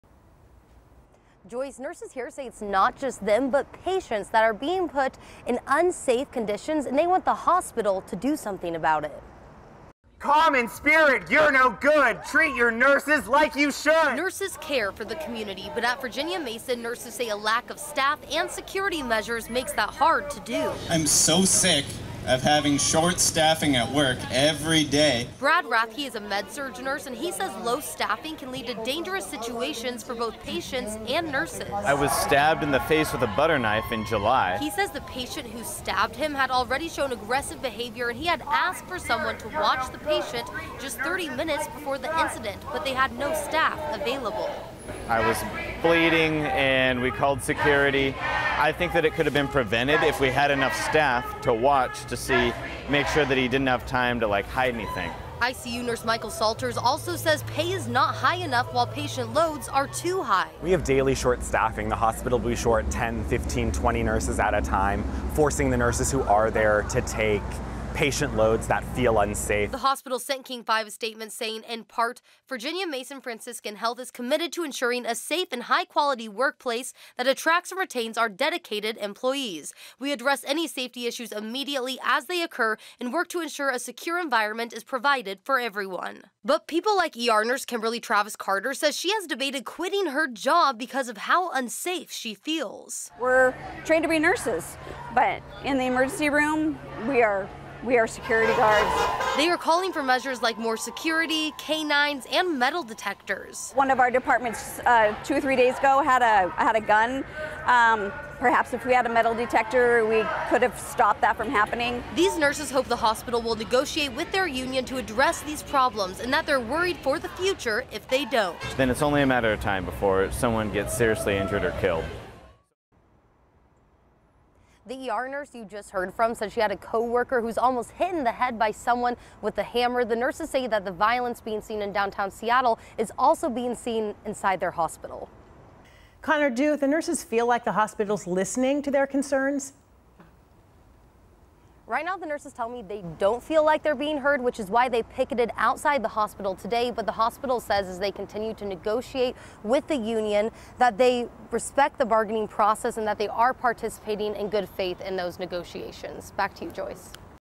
Nurses at Virginia Mason picket over workplace violence, low staffing and wages
One nurse was stabbed in the face with a butter knife and another missed being hit in the head with a claw hammer, according to the union.